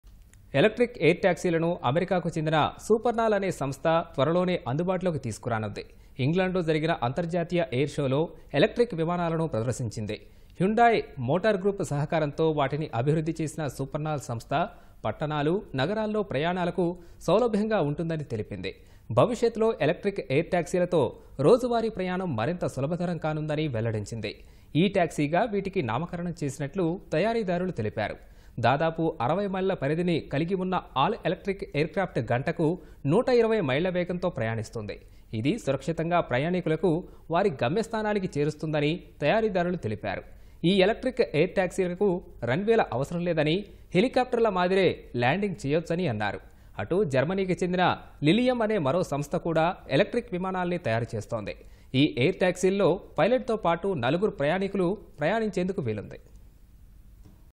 త్వరలో అందుబాటులోకి ఎలక్ట్రిక్ ఎయిర్ ట్యాక్సీలు | Electric Air Taxis in U.S
ఎలక్ర్టిక్  ఎయిర్  ట్యాక్సీలను అమెరికాకు చెందిన సూపర్నాల్  అనే సంస్థ...
త్వరలోనే అందుబాటులోకి తీసుకురానుంది. ఇంగ్లాండ్ లో జరిగిన...అంతర్జాతీయ ఎయిర్ షోలో ఎలక్ర్టిక్  విమానాలను ప్రదర్శించింది. హ్యుందాయ్  మోటార్  గ్రూప్  సహకారంతో వాటిని అభివృద్ధి చేసిన సూపర్నాల్  సంస్థ... పట్టణాలు, నగరాల్లో ప్రయాణాలకు సౌలభ్యంగా ఉంటుందని...తెలిపింది. భవిష్యత్తులో ఎలక్ట్రిక్  ఎయిర్  ట్యాక్సీలతో రోజువారీ ప్రయాణం మరింత సులభతరం కానుందని...వెల్లడించింది. E-ట్యాక్సీగా వీటికి నామాకరణం చేసినట్లు...తయారీదారులు తెలిపారు. దాదాపు
60-మైళ్ల పరిధిని...కలిగి ఉన్న ఆల్-ఎలక్ట్రిక్ ఎయిర్ క్రాఫ్ట్ గంటకు 120మైళ్ల వేగంతో ప్రయాణిస్తుంది. ఇది సురక్షితంగా ప్రయాణికులను...వారి గమ్యస్థానానికి చేరుస్తుందని తయారీదారులు తెలిపారు. ఈ ఎలక్ట్రిక్  ఎయిర్  ట్యాక్సీలకు రన్ వేల అవసరం లేదని... హెలికాప్టర్ల మాదిరే ల్యాడింగ్  చేయవచ్చన్నారు. అటు జర్మనీకి చెందిన లిలియం అనే మరో సంస్థ కూడా ఎలక్ర్టిక్  విమానాలను తయారు చేస్తోంది. ఈ ఎయిర్  ట్యాక్సీల్లో పైలట్ తోపాటు నలుగురు ప్రయాణించేందుకు వీలుంది